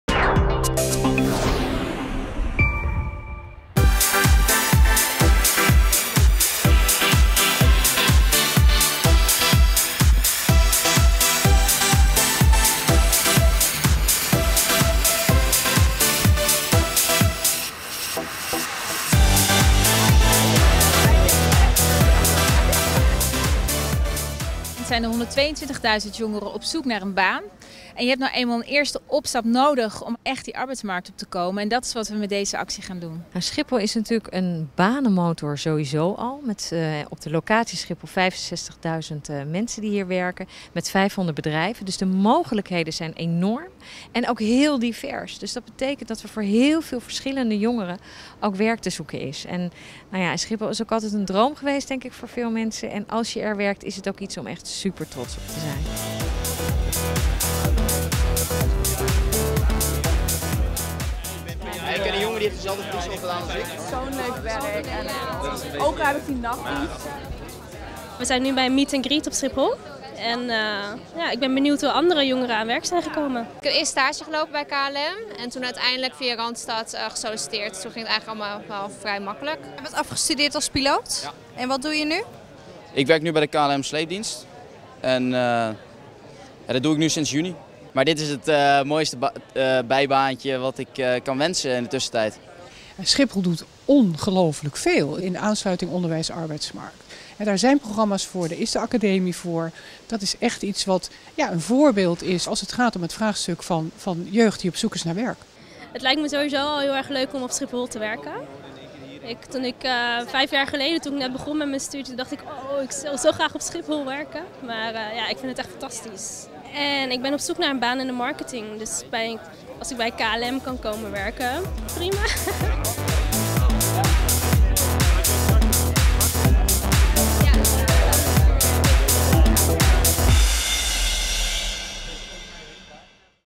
Jeugd op zoek naar werk op Schiphol
Met het het programma Jeugd op Zoek vraagt Randstad aandacht voor jeugdwerkloosheid. Schiphol doet veel om jongeren te helpen aan werk op de luchthaven. Daarom ging op 3 september het programma op Schiphol van start.